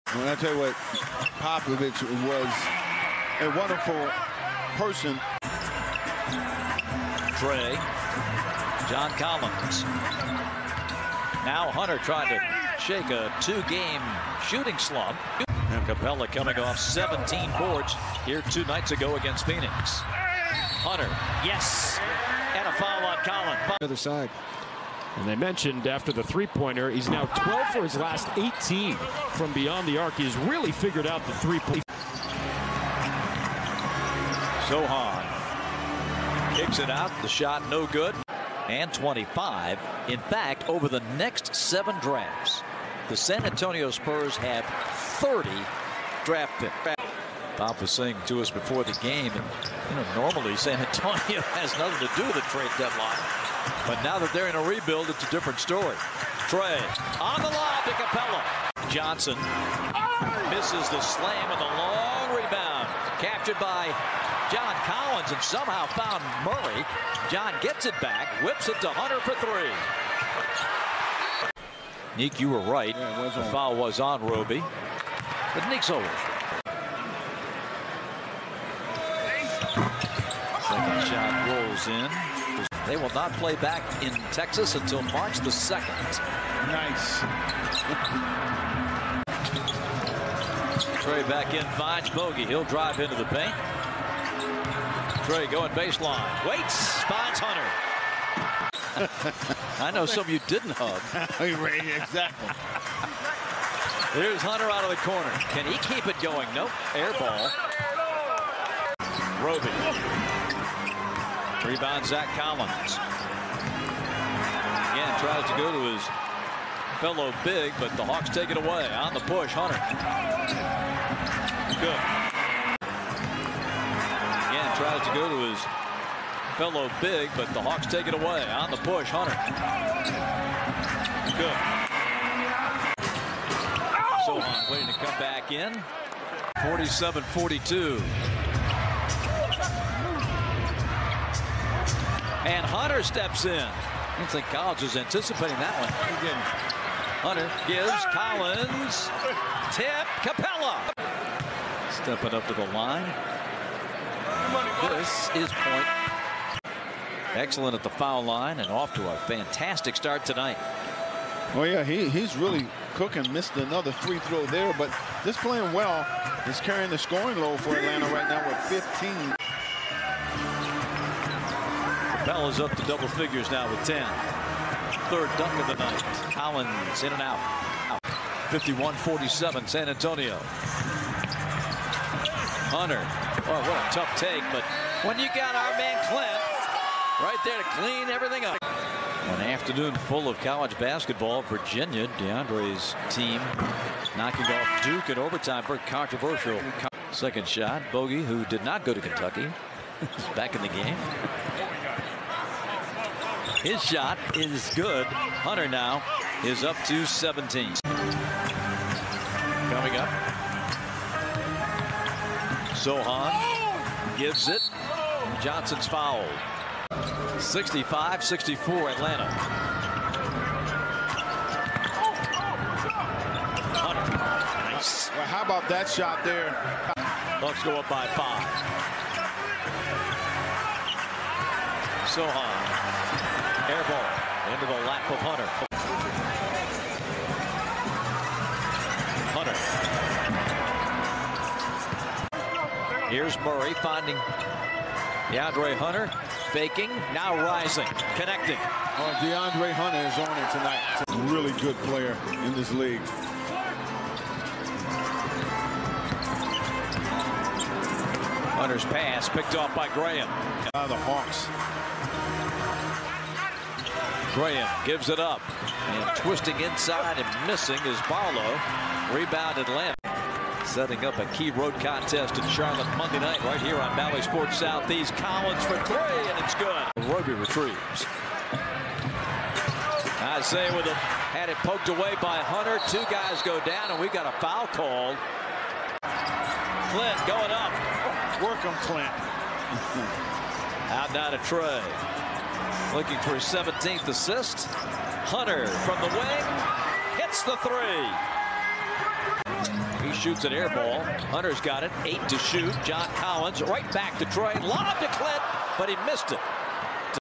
De'Andre Hunter 24 PTS: All Possessions (2023-02-11)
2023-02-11 Atlanta Hawks vs San Antonio Spurs: Player Highlights De'Andre Hunter
    24 PTS, 5 REB, 1 AST, 3 STL.
        2 of 5 from 3.
        8 of 13 FGs in 32:51 minutes.